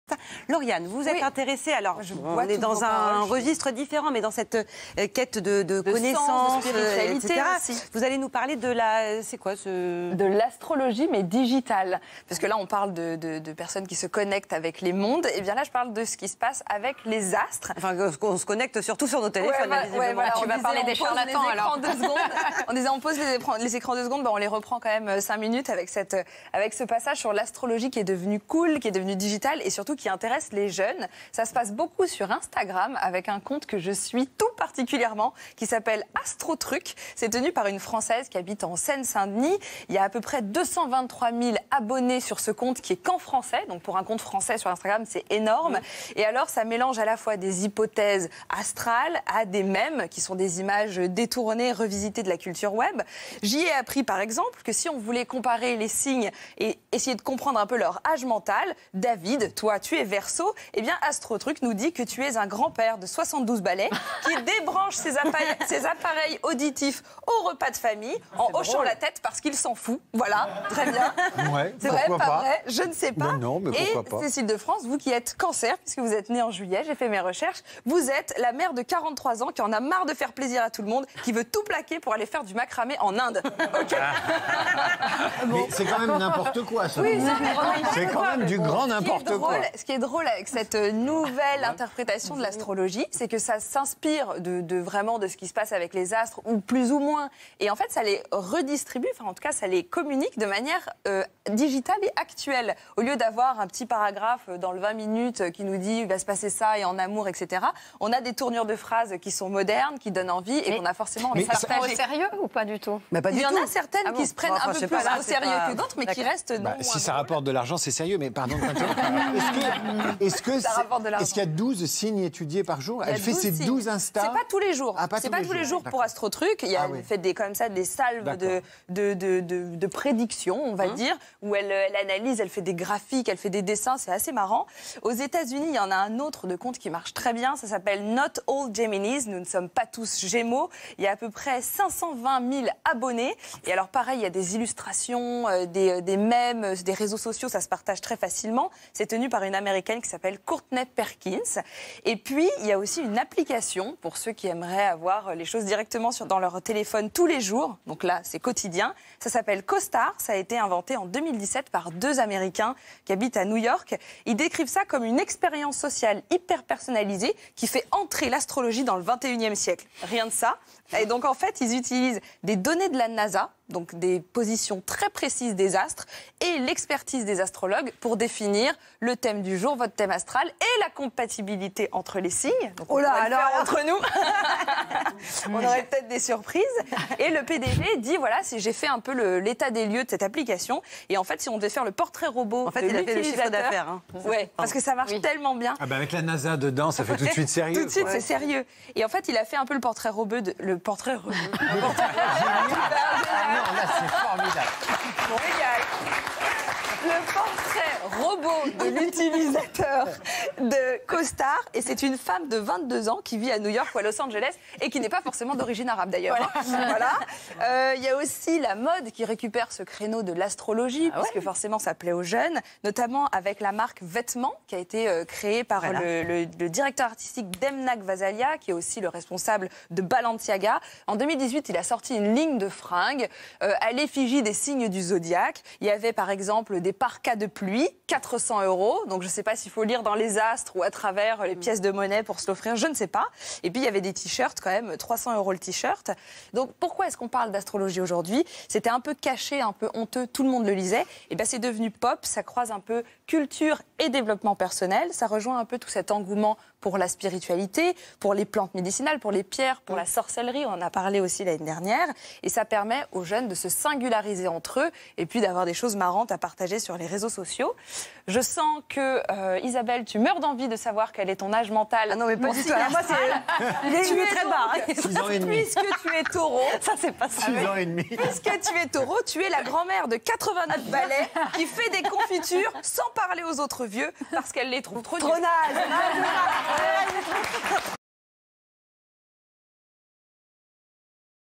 L'astrologie digitale avec Laurianne Melierre
Vous voulez savoir ce que révèle votre signe astrologique sur vous ? Aujourd'hui, en faisant un simple clic sur une application, vous pouvez découvrir ce que les astres réservent à votre avenir